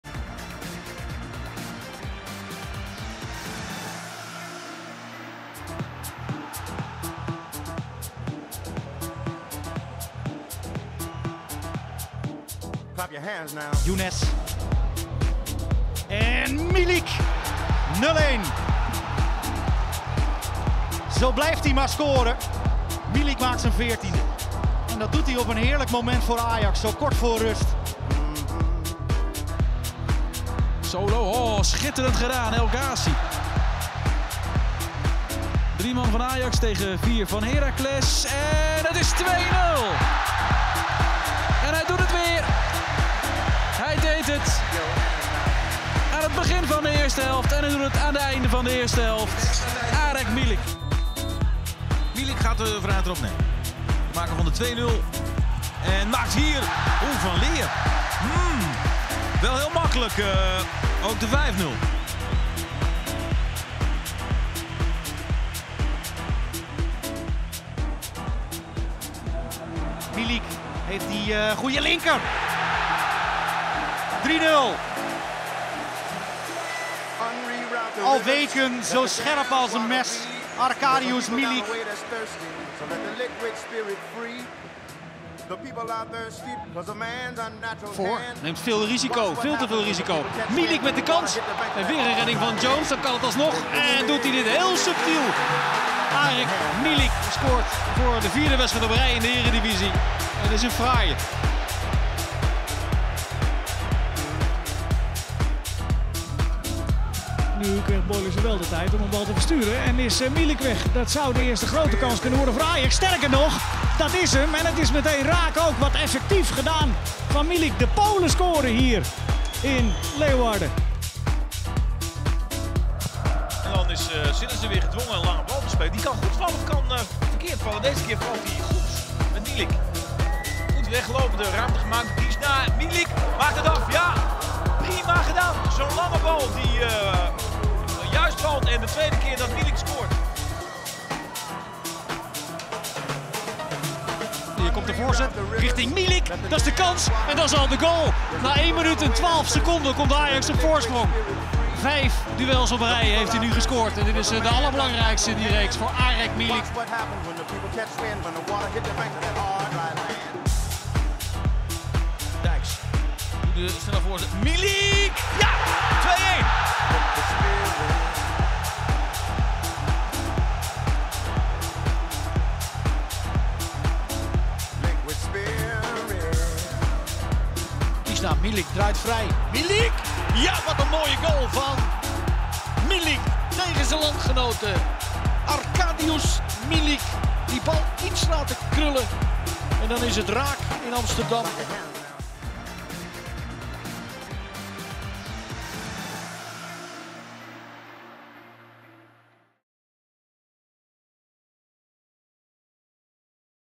TOP 10 GOALS - Arek Milik
De tien mooiste doelpunten van Arek Milik in het shirt van Ajax.